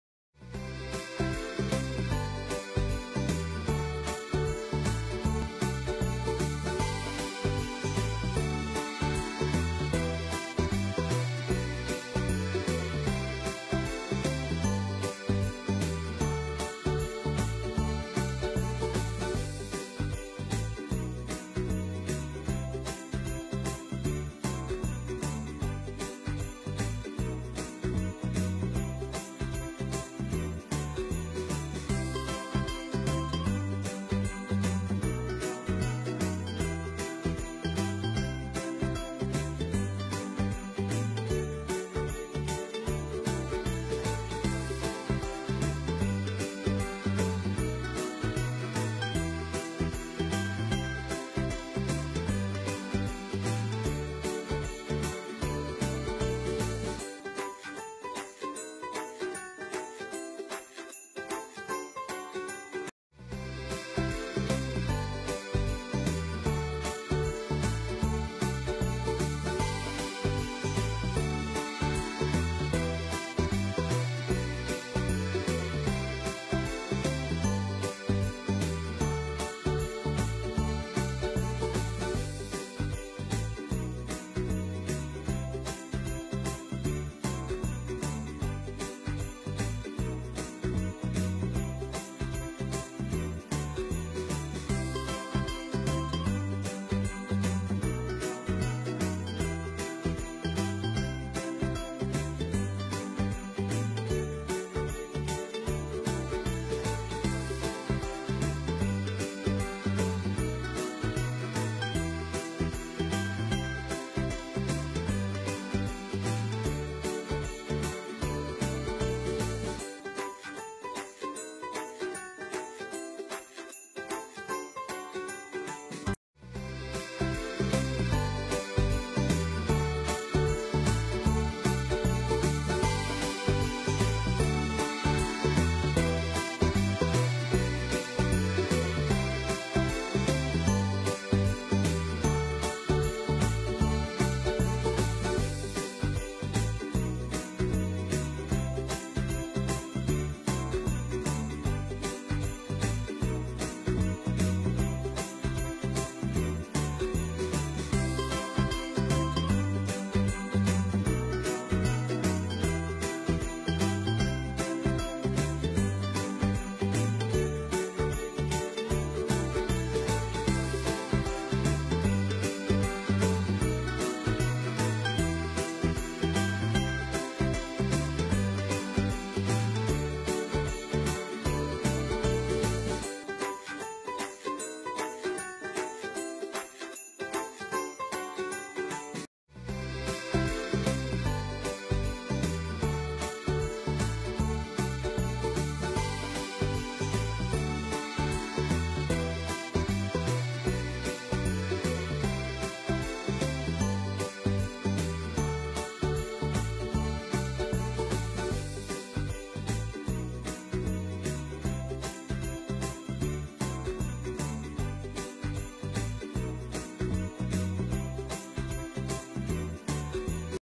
How To Make Flower Rangoli, Latest Flower Rangoli Design, Easy and Simple Rangoli Flower Ideas
Colorful floral rangoli designs with petals, flowers and colors for wedding, poojas, diwali, new year and any occasion to decorate and beautify your home.